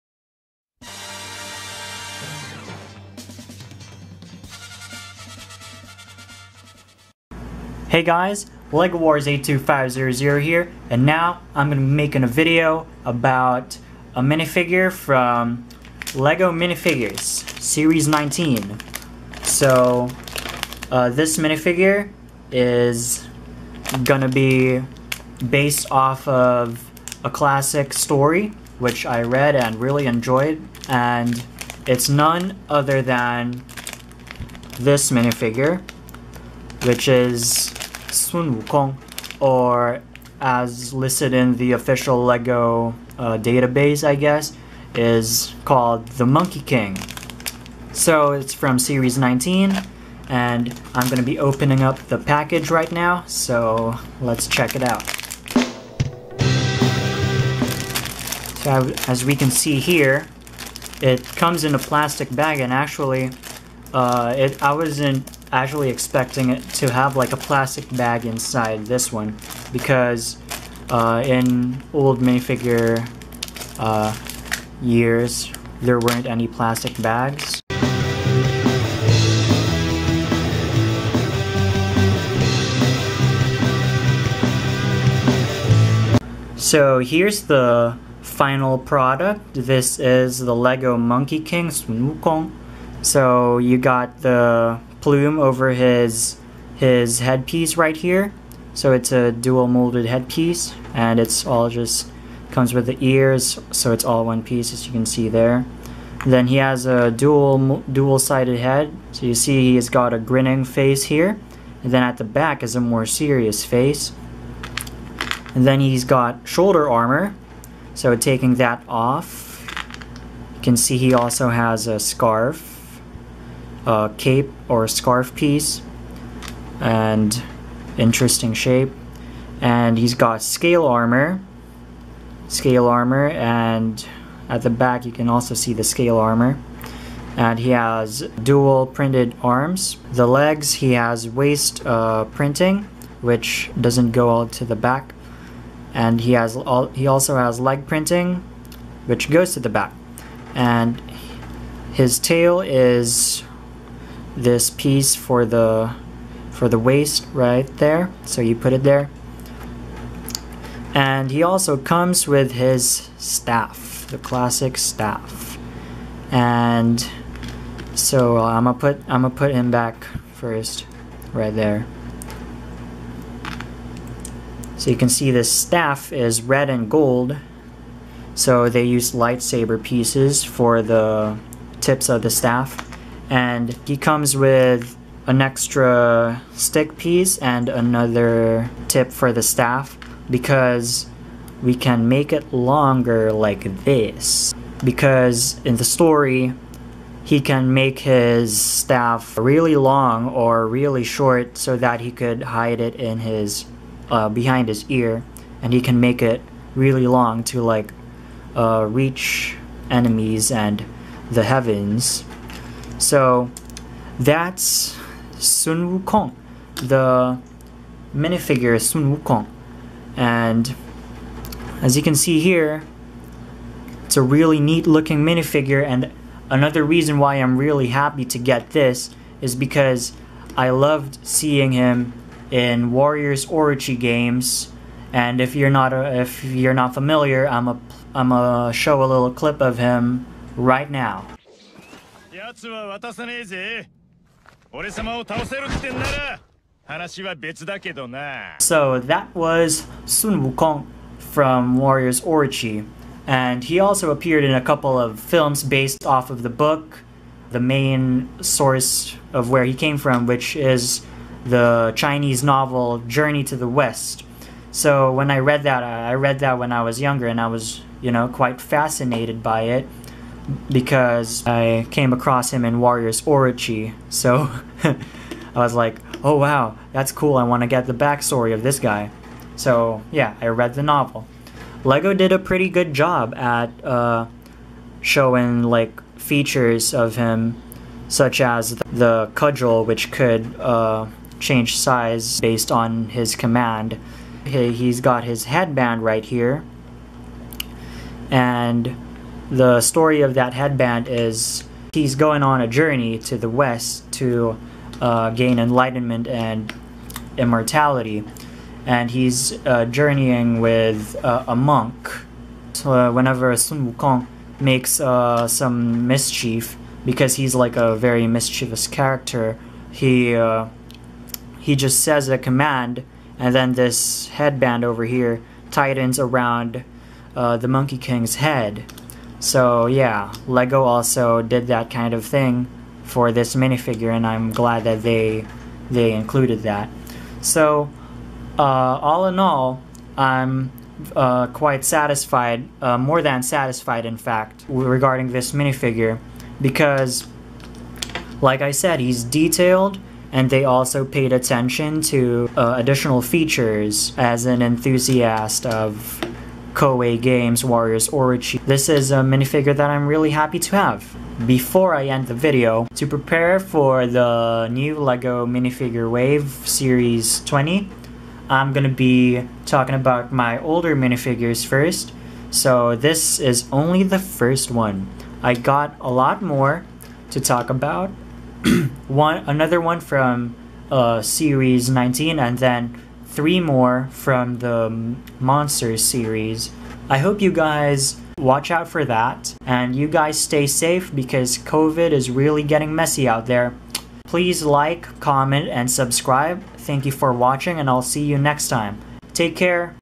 LEGO Sun Wukong! Monkey King Minifigure (Collectible Minifigures Series 19)
This is Sun Wukong, but in LEGO form! Officially, the minifigure called the Monkey King in Collectible Minifigures Series 19, but still, I wanna call it Sun Wukong :3 Enjoy and stay safe amidst this COVID-19 pandemic!